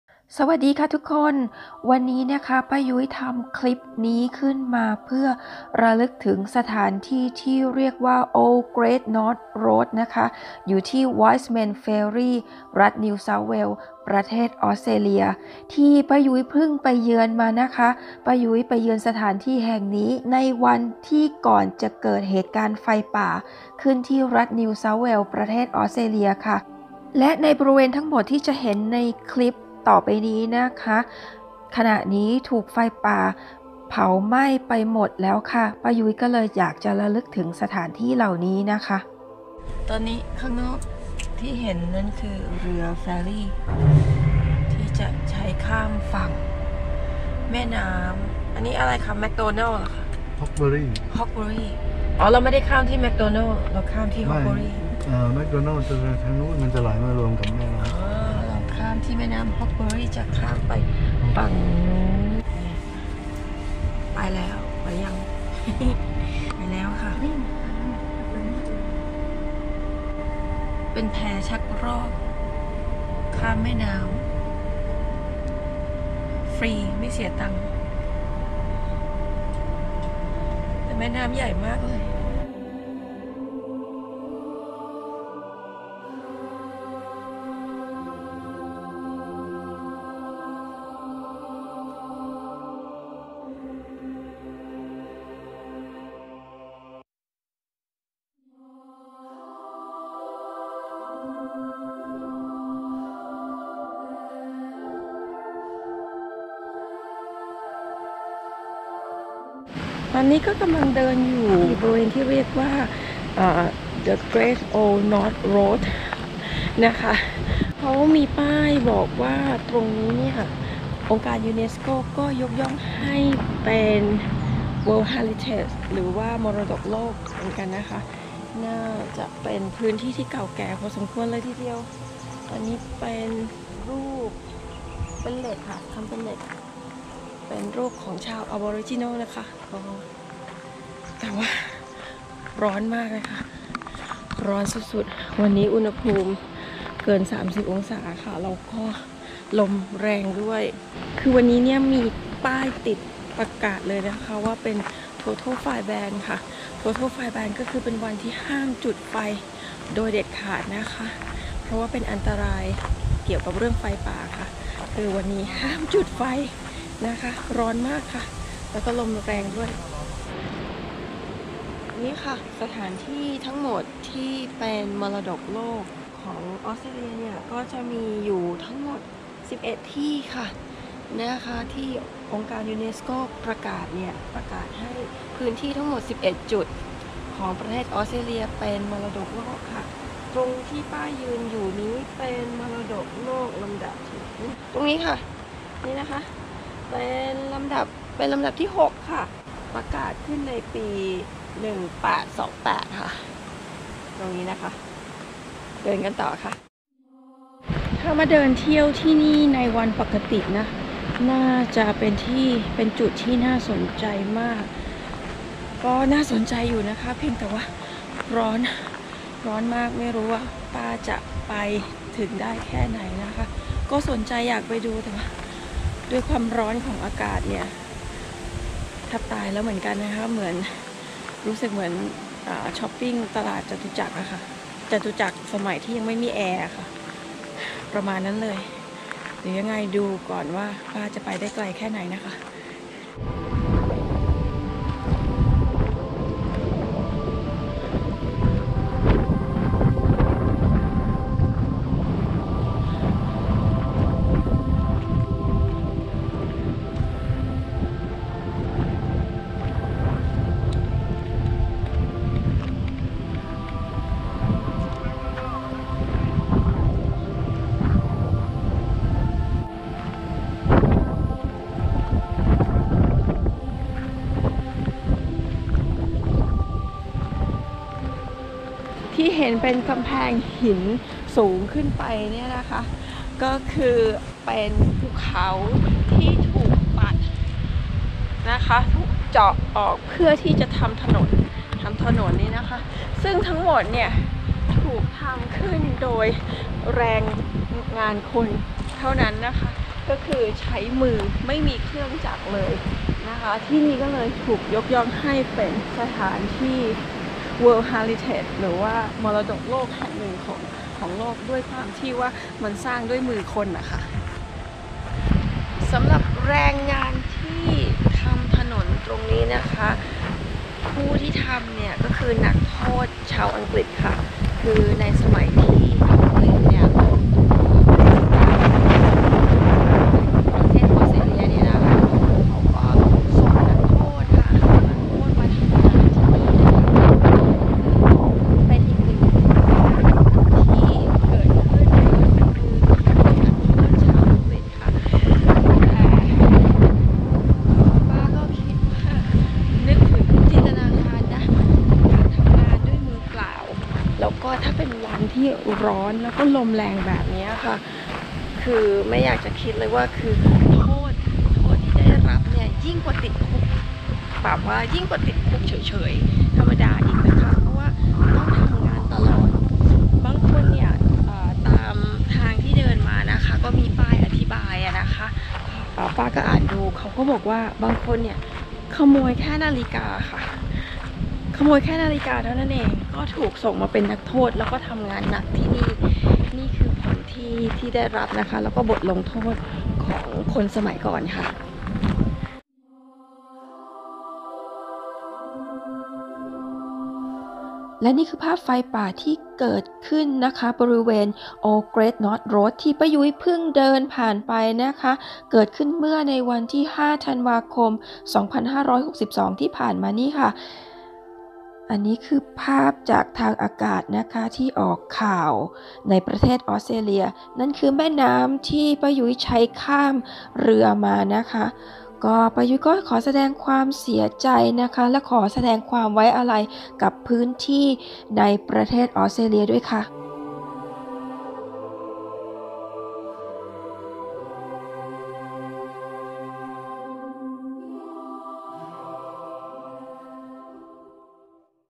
ถนนสายมรดกโลก Old Great North Road ในวันก่อนที่จะเกิดไฟป่าออสเตรเลีย / ป้ายุ้ยออสเตรเลีย
Old Great North Road เป็นถนนสายที่ได้รับการยกย่องจากองค์การยูเนสโก ให้เป็นพื้นที่มรดกโลก ตั้งอยู่ในเขตอุทยานแห่งชาติบูลเมาเท่น รัฐนิวเซาท์เวลส์ เป็นถนนสายที่ก่อสร้างขึ้นด้วยมือของนักโทษชาวอังกฤษ ซึ่งทำจากมือล้วนๆ ไม่มีเครื่องจักรใดๆเลย เป็นสถานที่ทางประวัติศาสตร์ที่น่าสนใจมากแห่งหนึ่งในออสเตรเลีย แต่น่าเสียใจ บริเวณที่เห็นในคลิปทั้งหมดนี้ ถูกทำลายโดยอัคคีภัย ไฟป่าออสเตรเลียไปหมดแล้ว เมื่อวันที่ 5 ธันวาคม 2562 ที่ผ่านมา